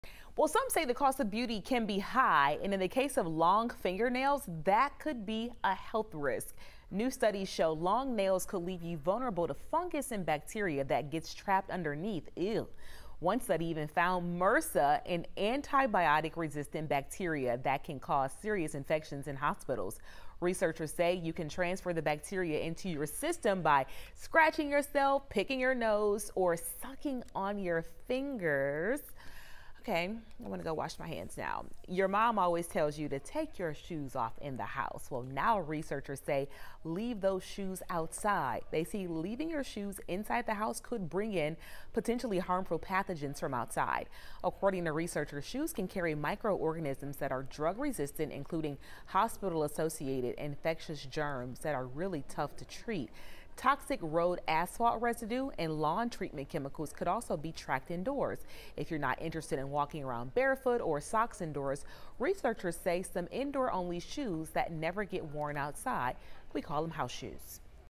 Long nails and leaving your shoes inside could produce harmful bacteria
Studies show that people with long nails can transfer the bacteria into their system and that wearing shoes indoors tracks the pathogens around the house.